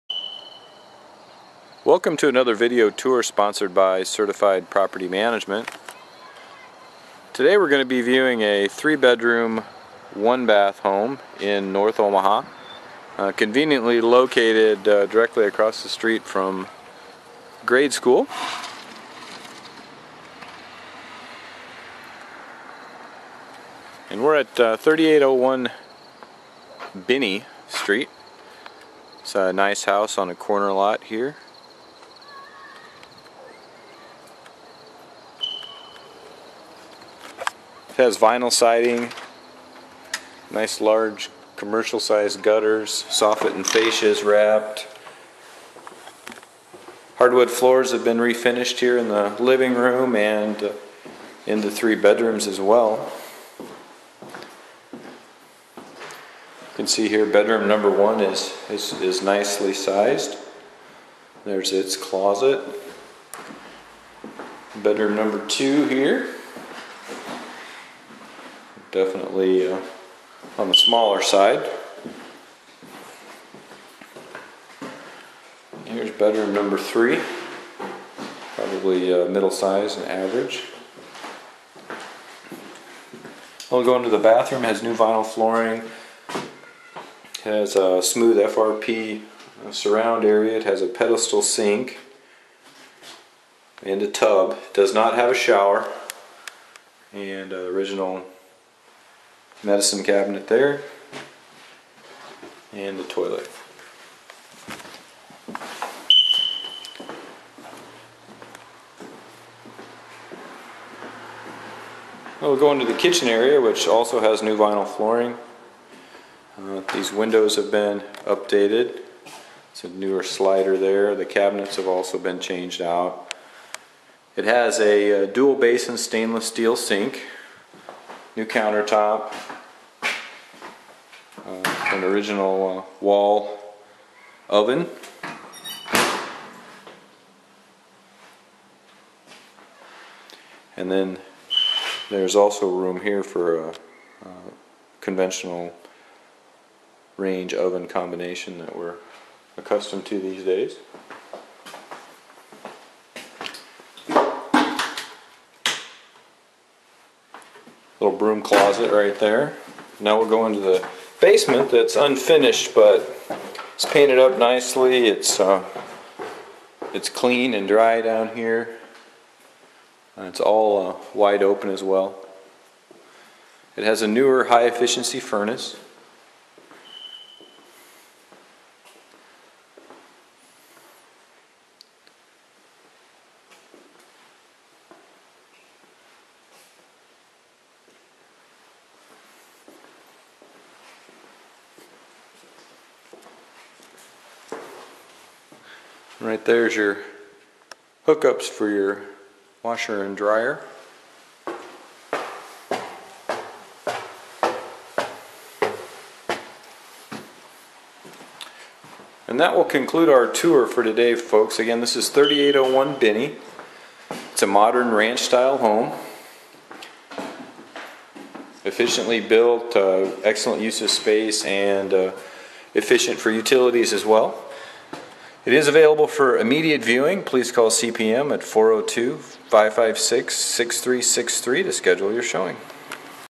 3801 Binney St 9-11
North Omaha, 3 Bedroom, 1 Bath, Ranch Style Home, Corner Lot, Across street from grade school